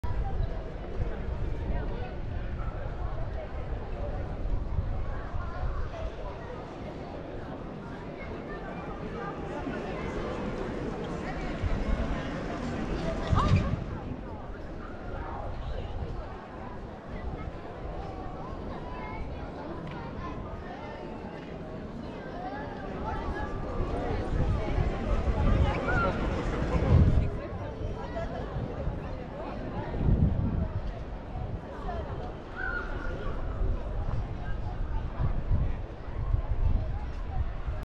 kids class at grand place
Cute video of a children's class traversing Grand Place